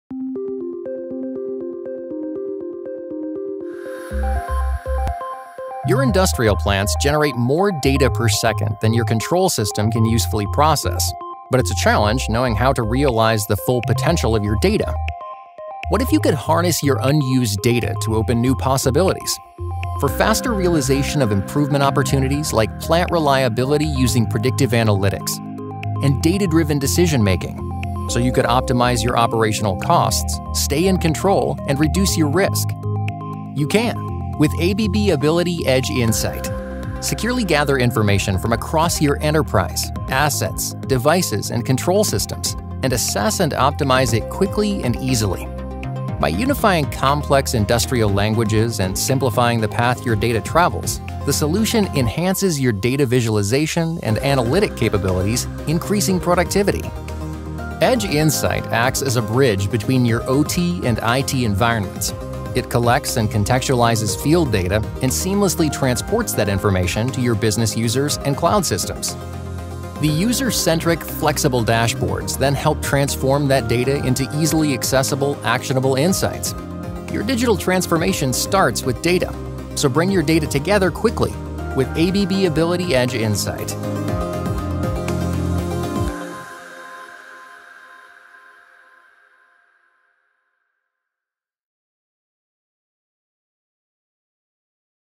ABB Ability™ EdgeInsight™
Optimize production by unleashing and leveraging real-time data from all of your OT devices